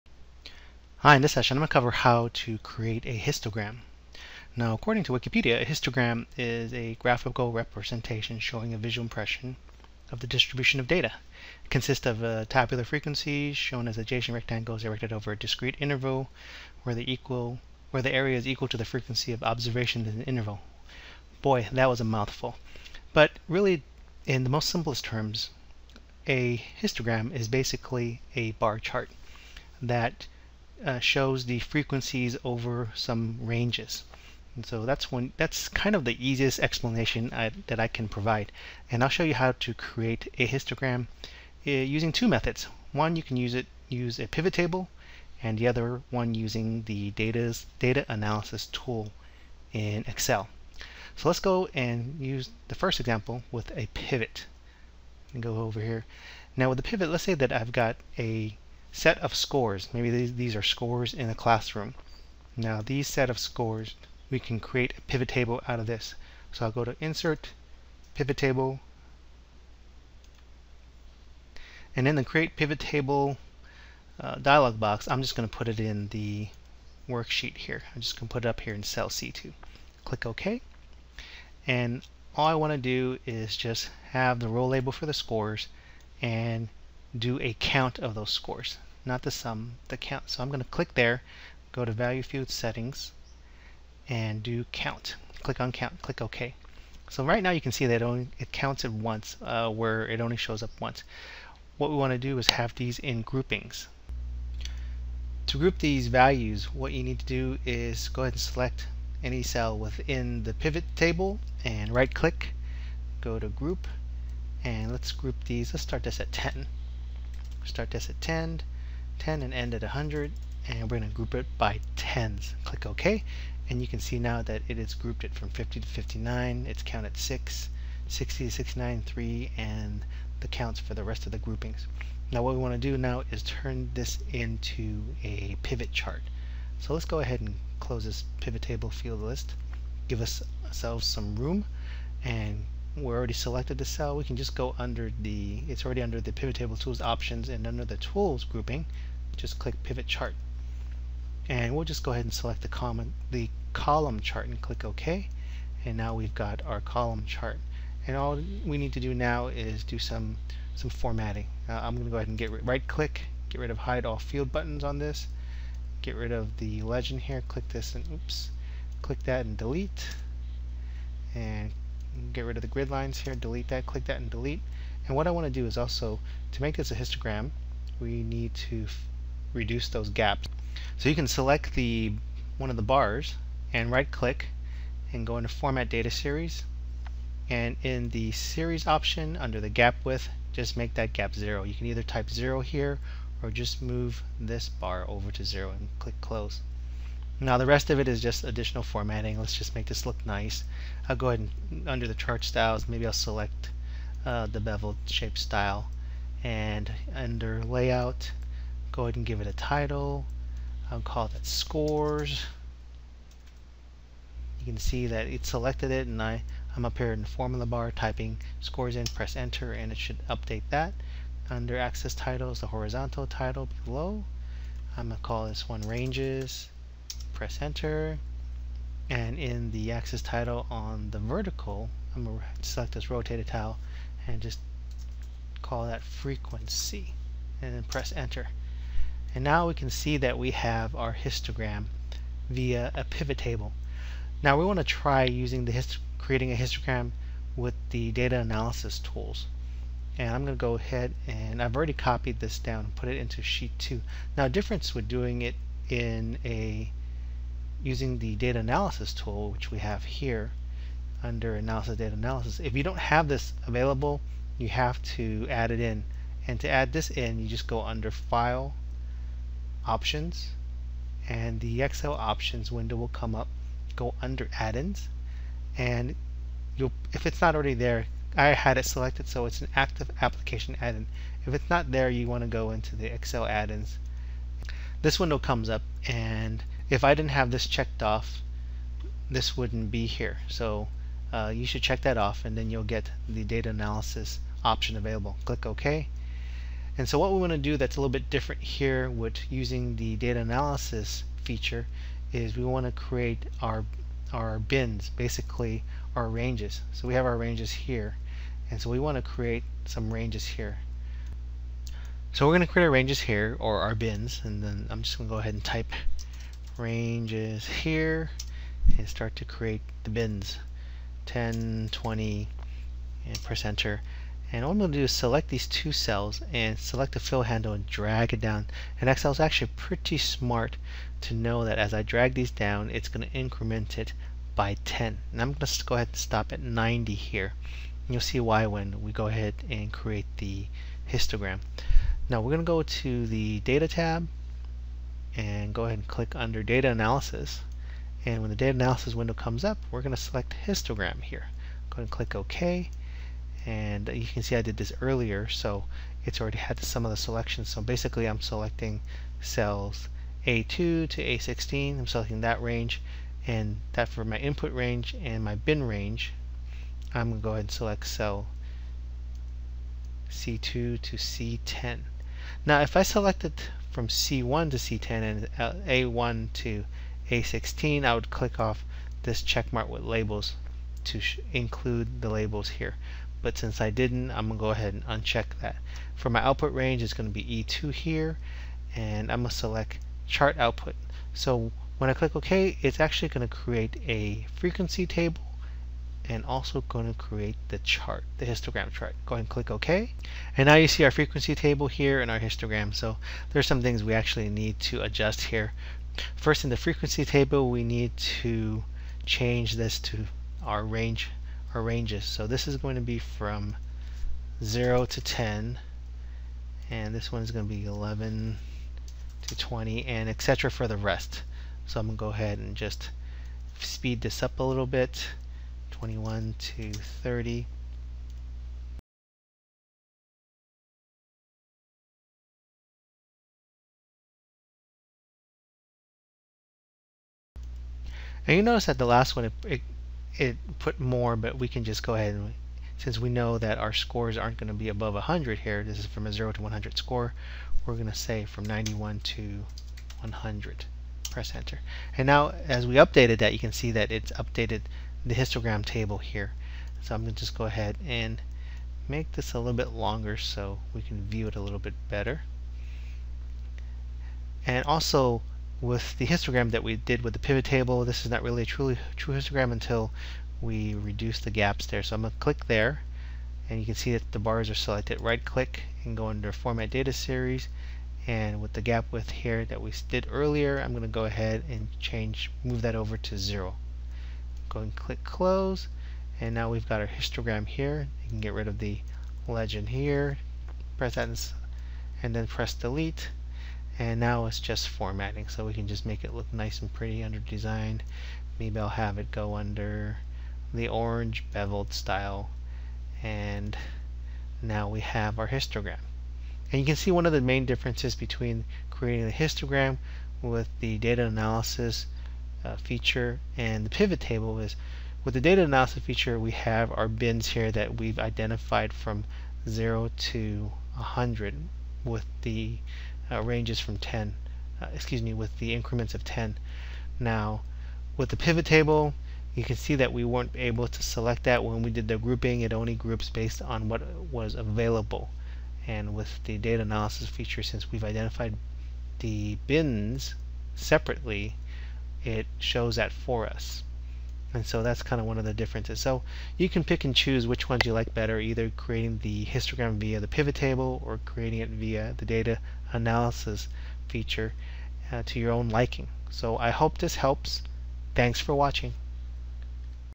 Create a Histogram
Histogram are often used in statistical analysis.  They're simple tools that help you visualize grouping of data.  Excel makes it super easy to create these cool analytic tools.  This is a basic introduction on how to create a histogram.  It will cover two methods (1) with PivotTable & PivotChart (2) with the Data Analysis Histogram feature.